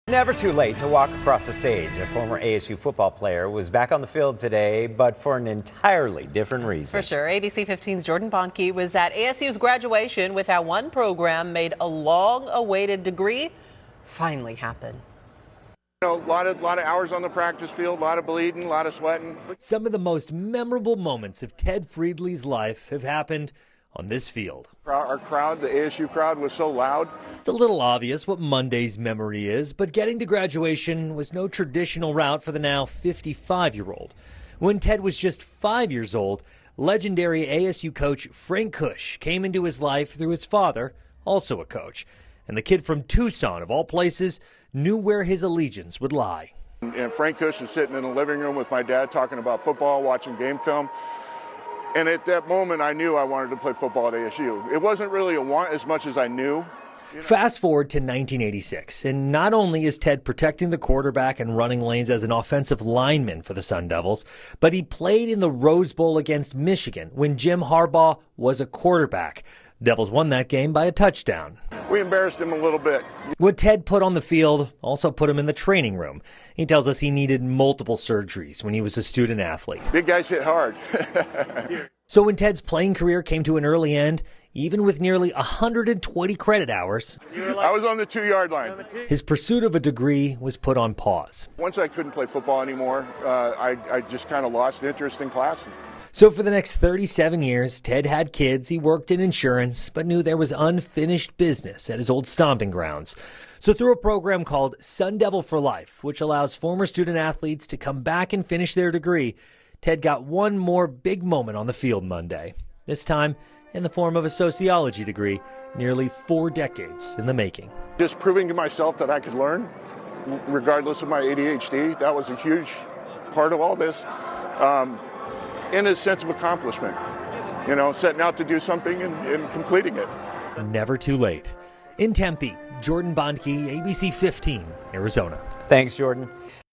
Former ASU football player Ted Friedli graduates 37 years after injury
Ted Friedli had a successful career at Arizona State University playing football for the Sun Devils for three seasons. Unfortunately, multiple injuries and social distractions, he said, led to declining grades, causing Friedli to eventually take a step back from school and football. During his life, ASU says Friedli has suffered a stroke and has gone through 19 injury-related surgeries. Despite that, he still carried with him the hope that someday he would earn his diploma.